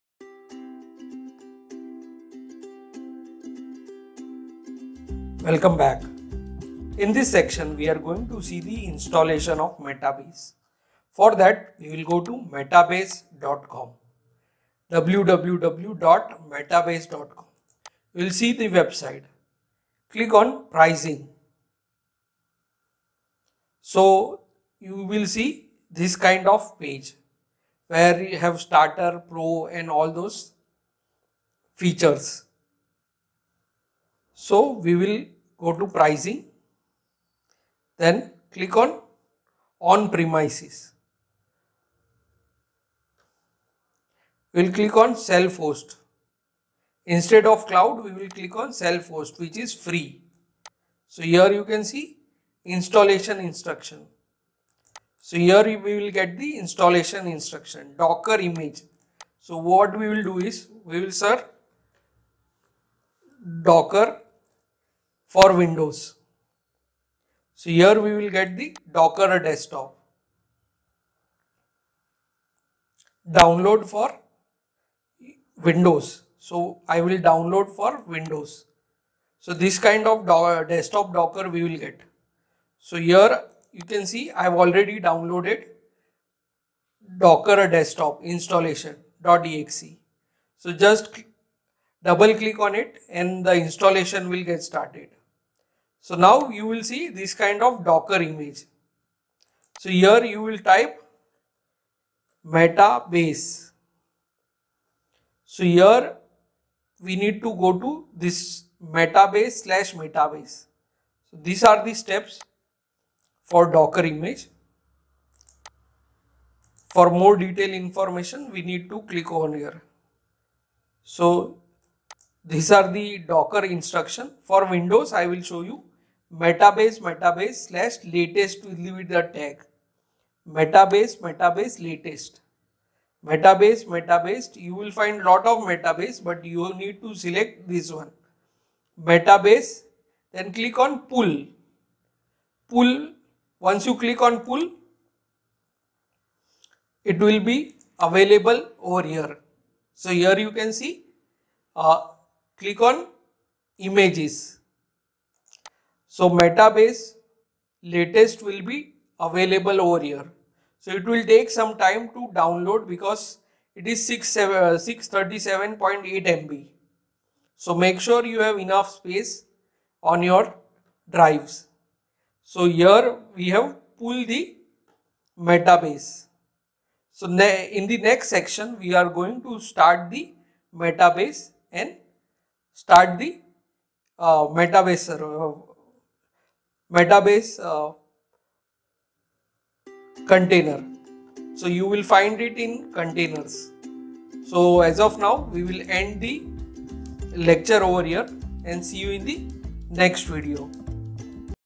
Installing Docker on Windows for Metabase (Hands-On)
"This is the first part of our step-by-step series on how to install and run Metabase on a Windows machine. In this video, we'll cover the essential prerequisites: navigating to the Metabase website, downloading Docker Desktop, and pulling the official Metabase Docker image. Get ready for the next part, where we will start the Metabase container!

What you will learn:
How to find the free self-hosted option for Metabase
The importance of Docker Desktop for the installation
How to search for and pull the Metabase Docker image from Docker Hub
Verifying your download and preparing for the next steps